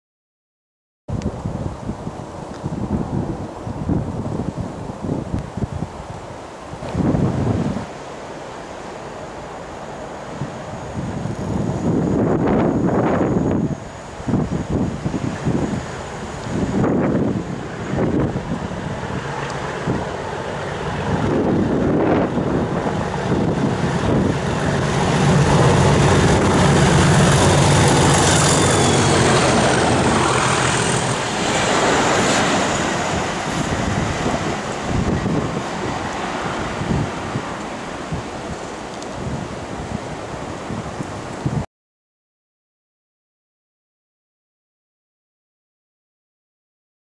(HD) WCRC 37516 + 47245 thrashes through Bearsted working the 8Y98 SITT 12.3.13
I have been waiting a while to see the SITT and I have finally seen it with a pair of WCRC beasts. Here we see 37516 clagging like hell with the SITT and 47245 in tow working the 8Y98 Tonbridge West Yard to Tonbridge West Yard Via Swanley.